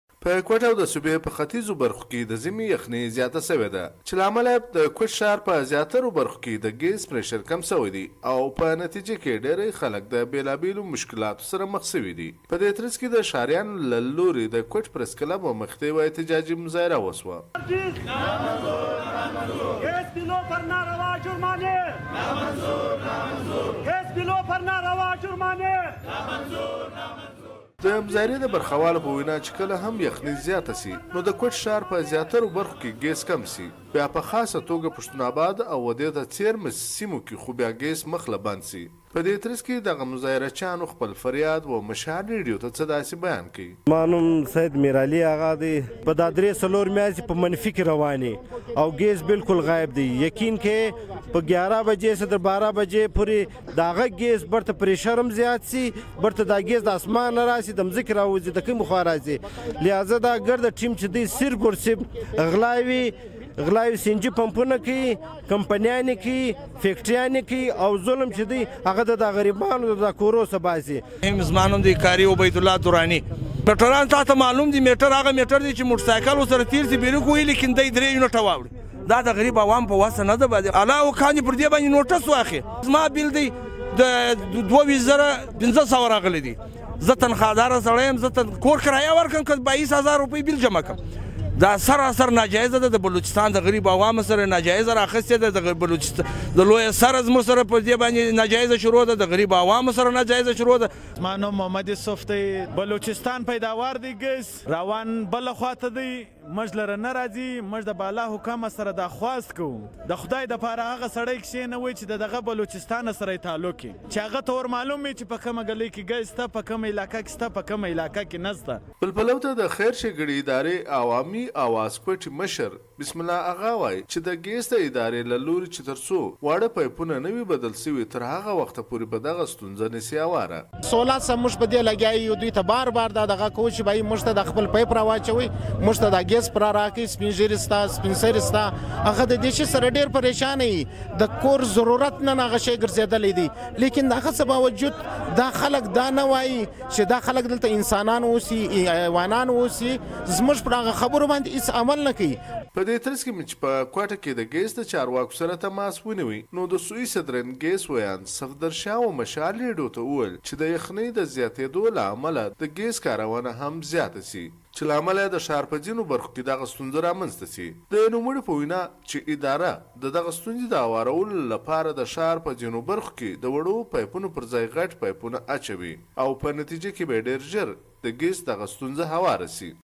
د کوټې اوسېدونکي له حکومته ګېس غواړي
د بلوچستان په مرکز کوټه کې د یخي زیاتېدو وروسته د ښار په ځینو برخو کې د ګېس فشار کم شوي دي چې له کبله یې خلک له ډېرو ستونزو سره مخامخ شوي. په دې اړه د ایوب ترین ویډیويي رپورټ وګورئ.